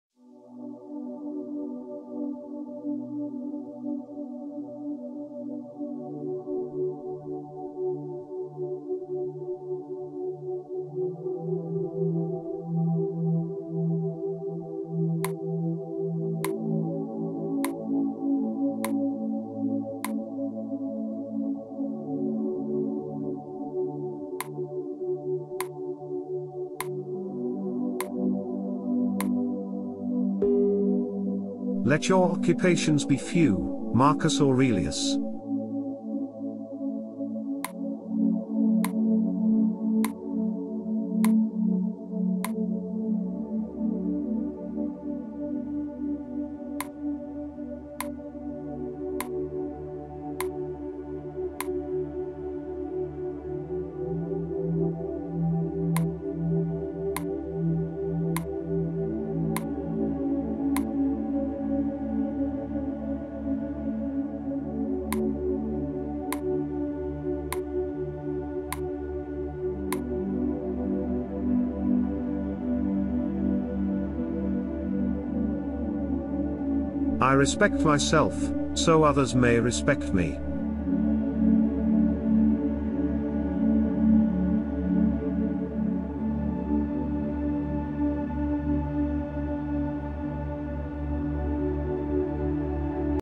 Daily Quote, Affirmation, guided mindfulness of the Day: Let your occupations...
Be in this moment

Quote:
"Let your occupations be few" -- Marcus Aurelius

Affirmation:
I respect myself, so others may respect me.

Source: Own work
Author: Basile Morin